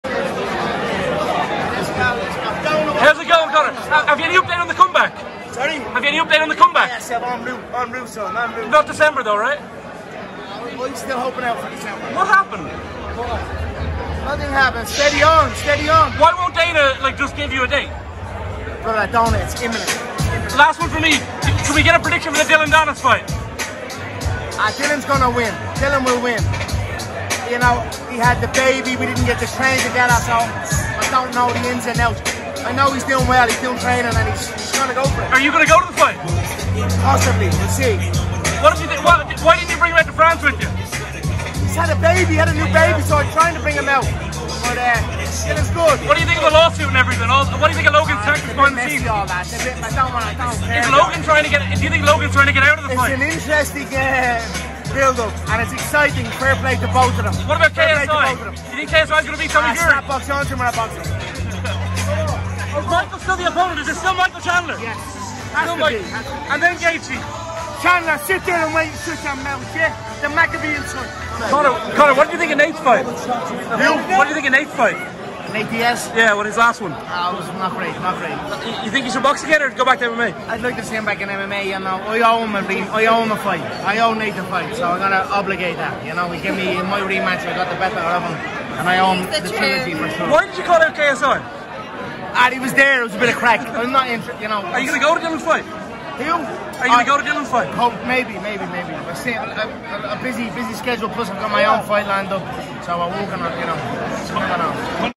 "WE DIDN'T TRAIN TOGETHER" - Conor McGregor on DILLON DANIS V LOGAN PAUL + KSI FEUD & UFC RETURN
Conor McGregor speaks exclusively with Mirror Fighting's Donagh Corby at the launch of his Forged Irish Stout, and talks about what's next for his career, Dillon Danis vs Logan Paul and much more.